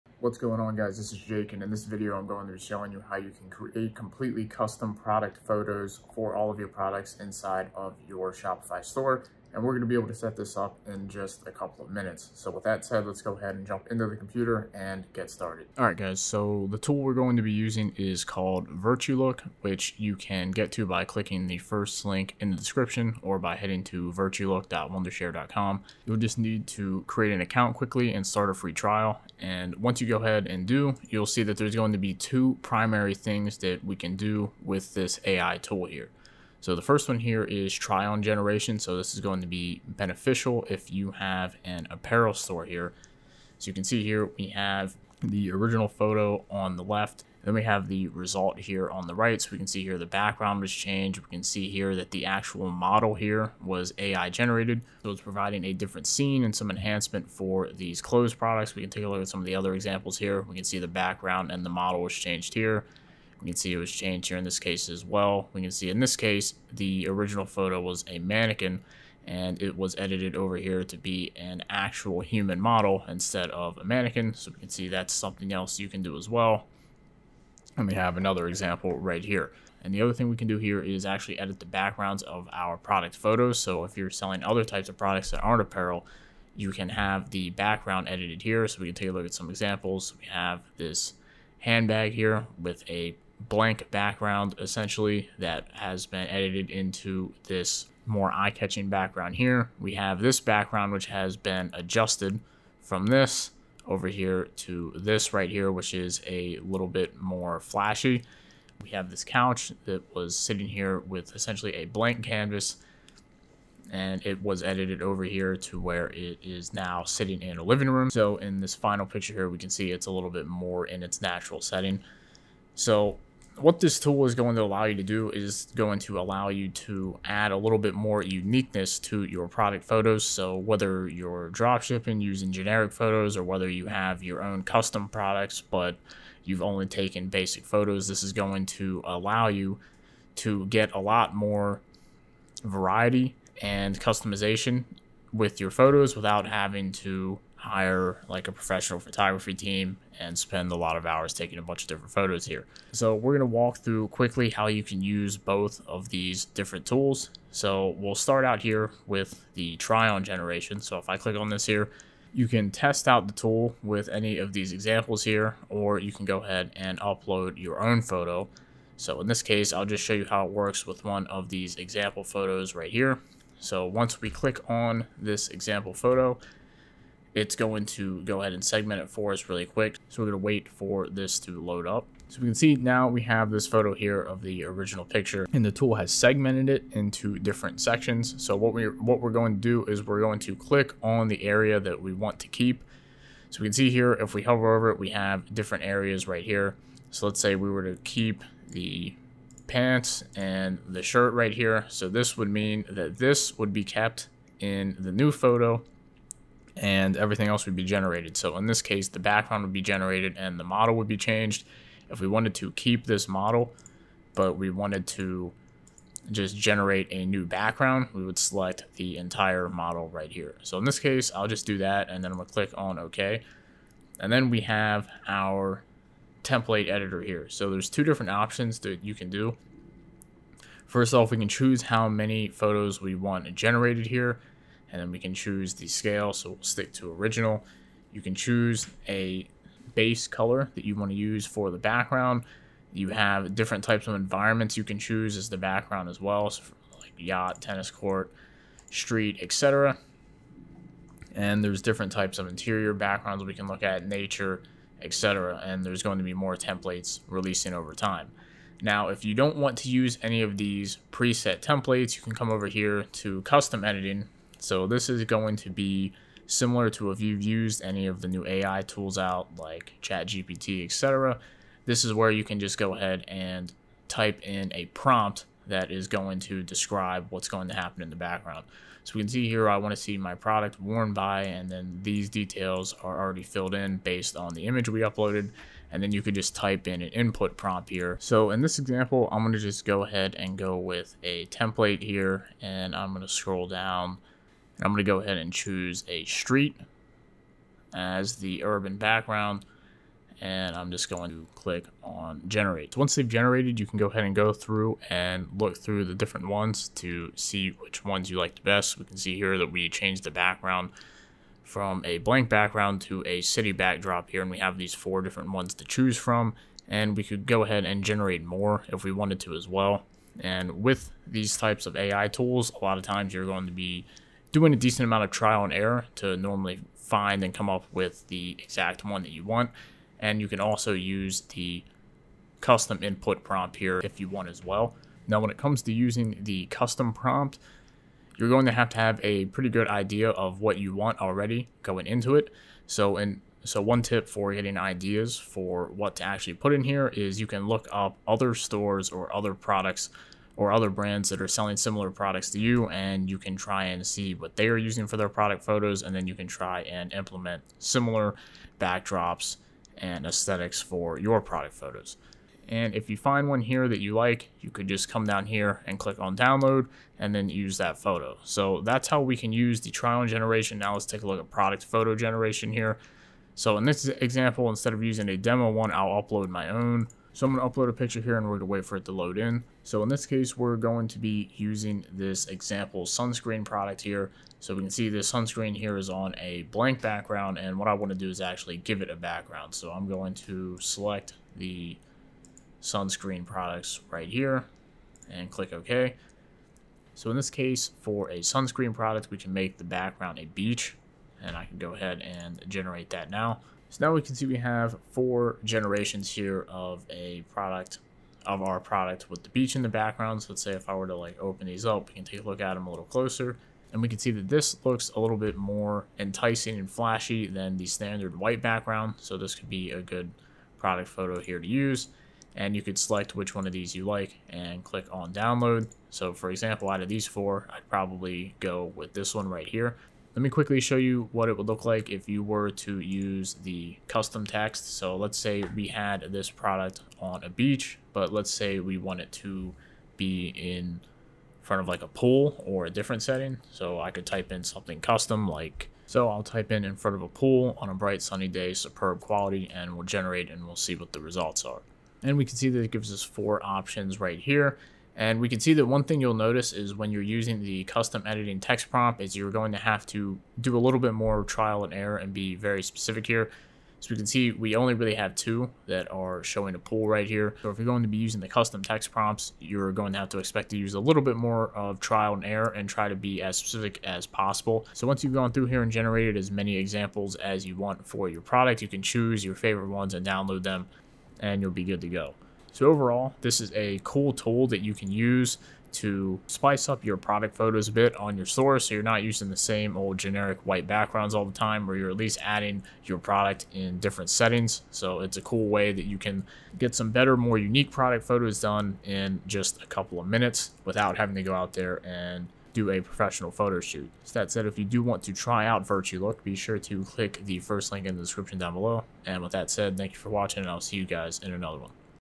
How To Create Quick Custom Product Images For Your Shopify Store Using AI
VirtuLook is an AI-powered image generator that empowers users to create stunning product photos using simple text or images, reducing image production costs, and boosting product conversion. In this video I will be showing you how you can use the Virtulook tool to create custom product images for your shopify products. This will help you to stand out and have unique product images without having to hire a professional photographer or have a photo shoot.